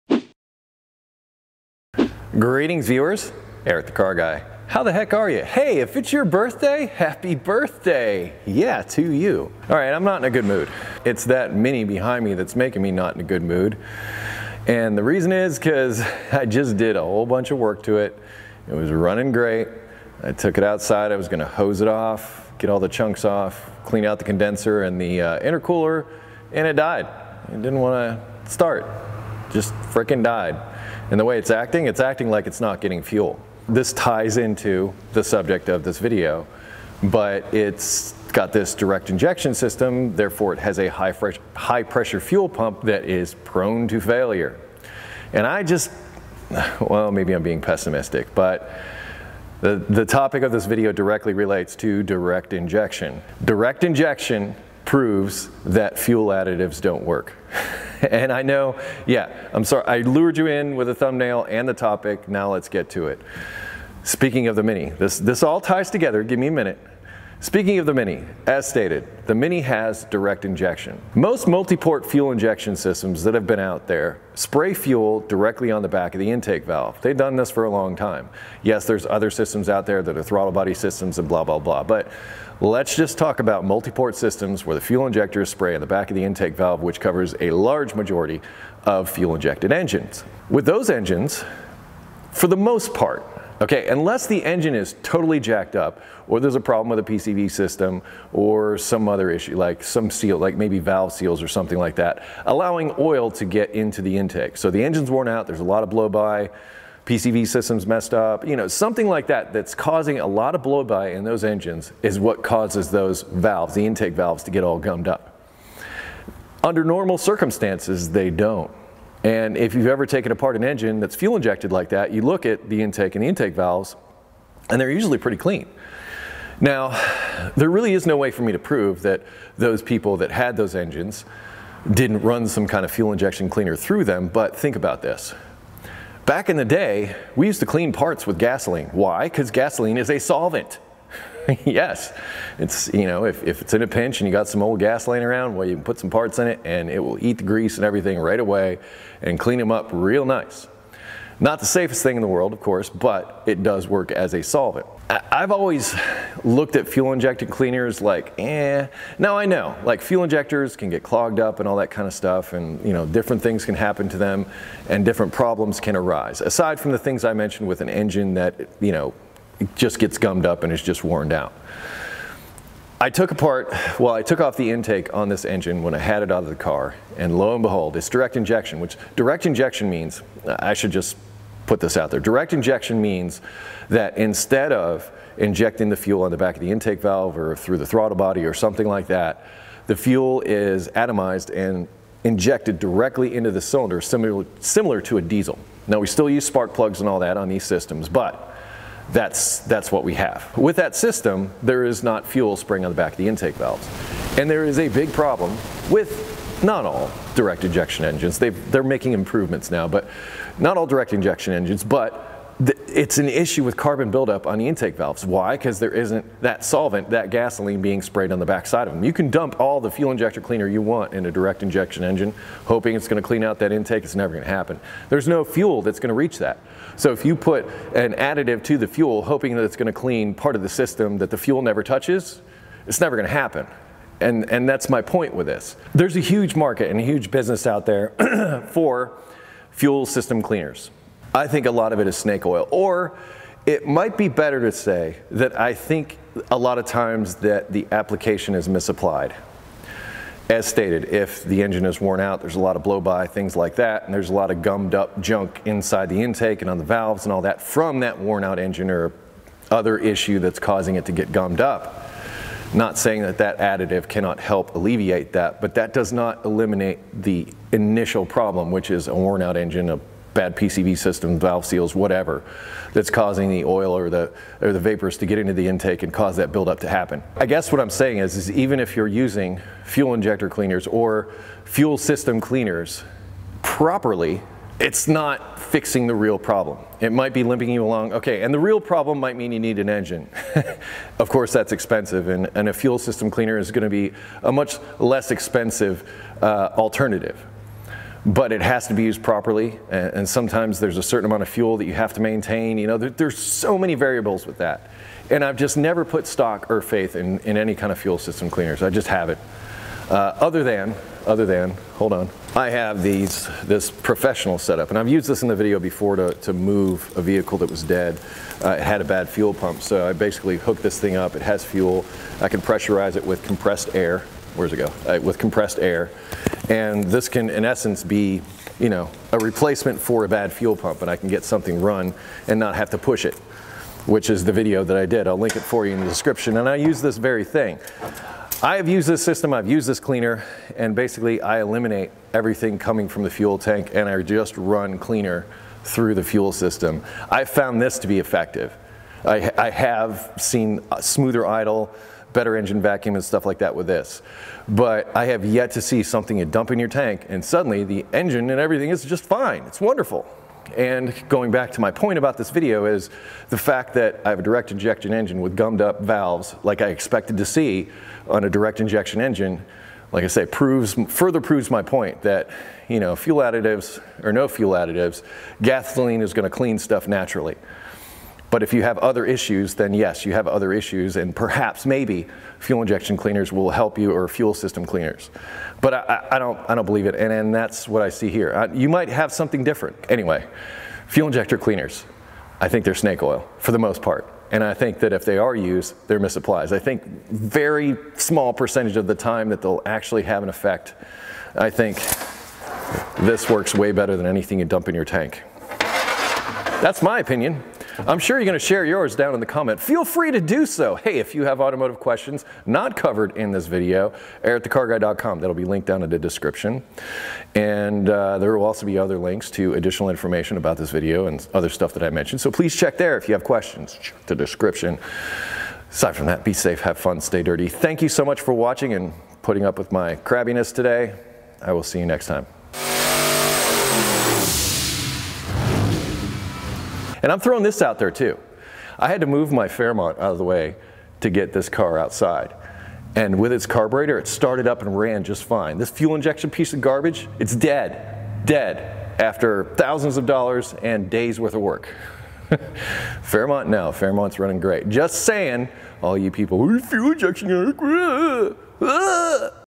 Direct Injection Proves Fuel Additives Don't Work
Recently I picked up a 2007 Mini Cooper S (R56) with direct injection. While doing other work on the engine, I decided to remove the intake to have a look at the intake valves. I found exactly what I expected, the valves were covered with performance robbing gunk.

I spent some time cleaning things up and it looks good as new now, but I have a feeling that one day this issue will come back again.

Why did this happen? 2 reasons. The first is direct injection. No fuel will ever make it to the back of the intake valve to clean it. Second, the N14 engine has a horrible PCV system. This means that the intake manifold is getting a constant supply of oily gunk that will eventually coat the intake valves again.

This is the foundation for my argument. If this car had a traditional injection system, the problem probably wouldn't be so bad.

I have other theories and explanations in the video.

Cleaning the Valves on the N14: Link posted when video is available.

Want more content from ETCG? Information on Premium Membership

Stay Dirty

ETCG1